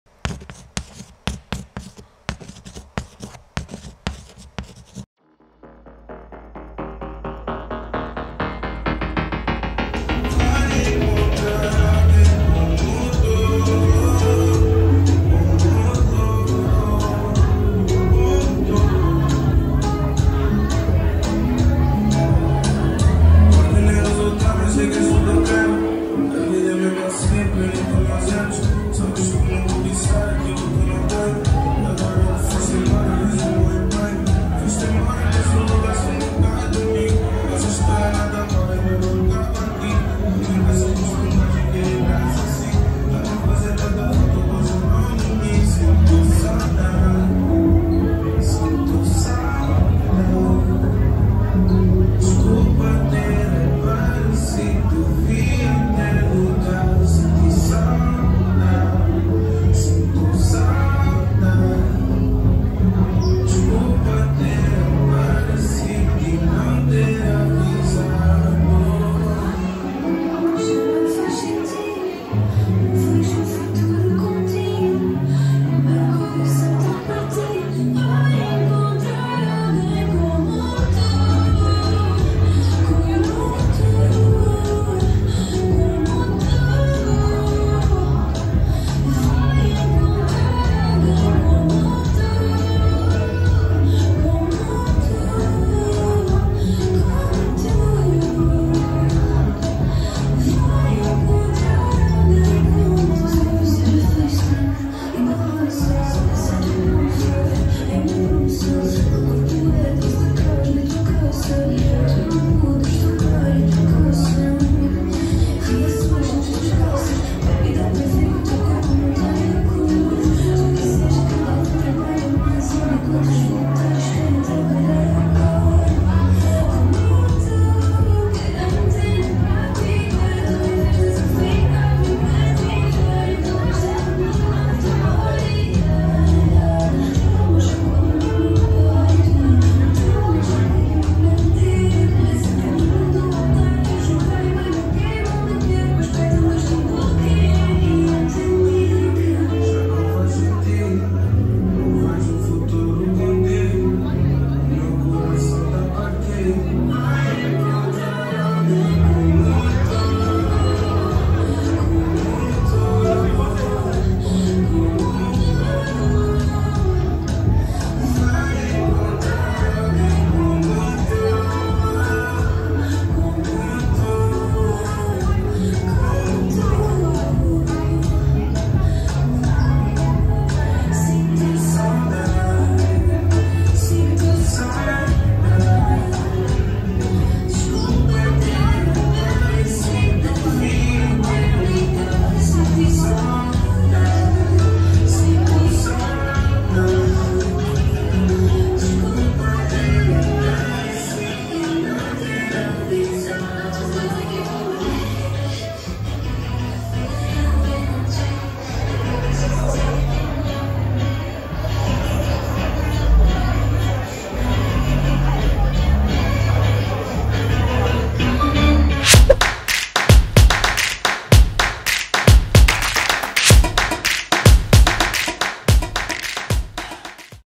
Filipe Monteiro & Alessandra Tavares (Zouk, Zouk Lambada) ZOUK IN RIO Sexta - 05 Jul 2024
Event: "Zouk in Rio" Baile de Sexta Feira
Dance: Zouk Brasileiro, ZoukLambada
Location: Nucleo de Dança Renata Peçanha, Lapa, Rio de Janeiro
Date: 05 Jul 2024